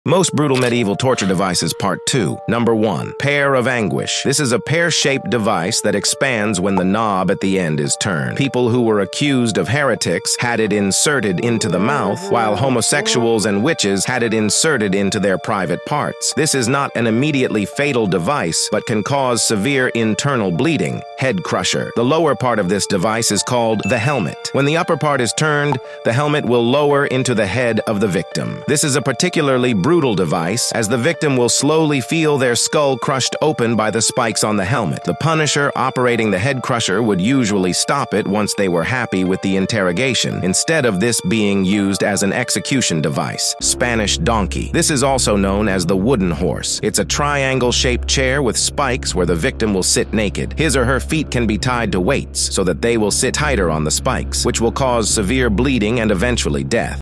Most brutal medieval torture devices part 2
Some of the most brutal medieval torture devices used in medieval times or dark ages.
ALL CREDITS ANIMATION: "Literacy Corner Global" on Youtube. You can check out a longer more informative video on their channel page.